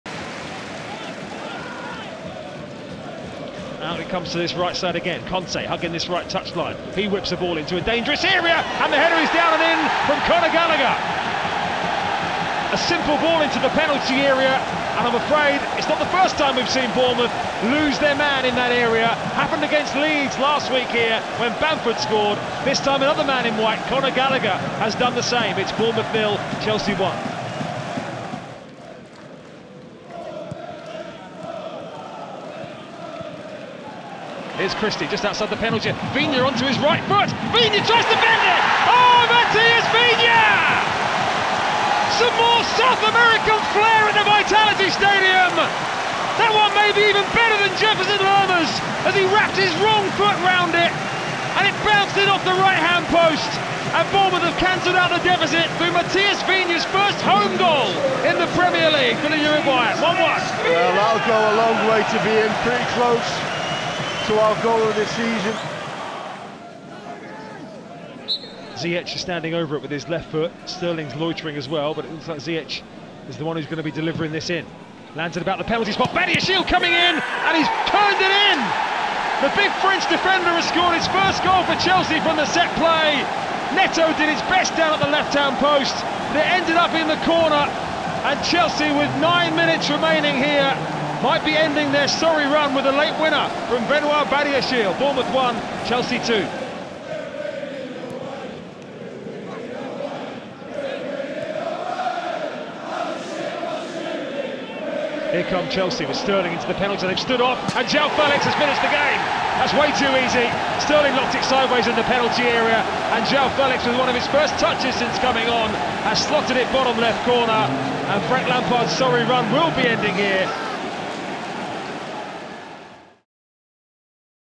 Viña nets worldie as the Blues earn victory | AFC Bournemouth 1-3 Chelsea
Matìas Viña scored a superb goal as AFC Bournemouth were defeated at home thanks to goals from Conor Gallagher, Benoît Badiashile and João Félix mean defeat.